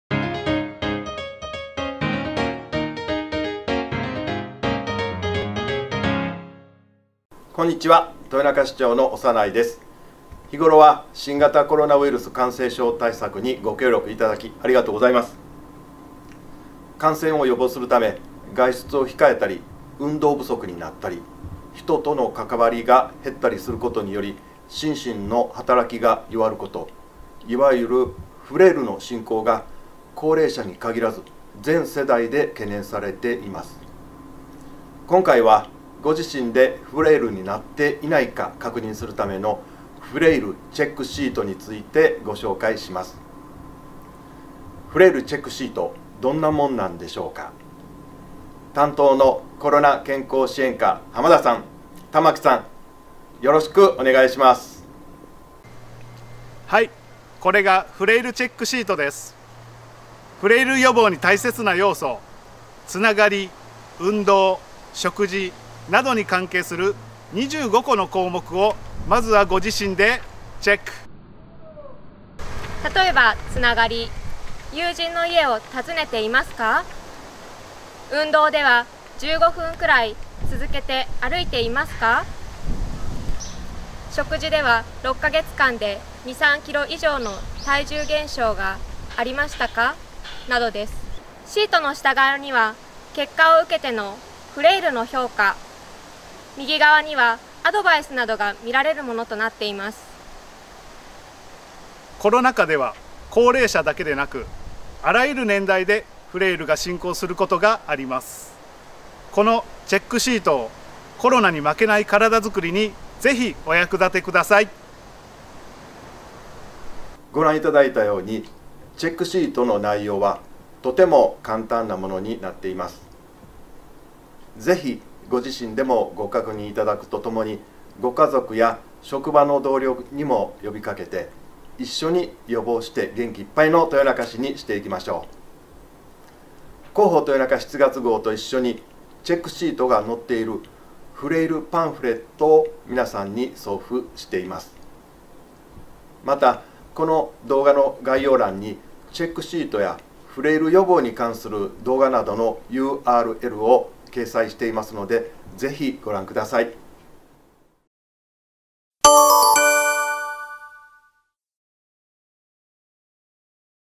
市民の皆さまへ~長内繁樹市長より≪コロナフレイルを予防しましょう≫~
コロナ禍における活動制限によって、心身の働きが弱る、いわゆるフレイルの進行が全世代で懸念されています。
ご自身の健康状態を確認できる「フレイルチェックシート」について、長内市長のメッセージとともにご紹介します。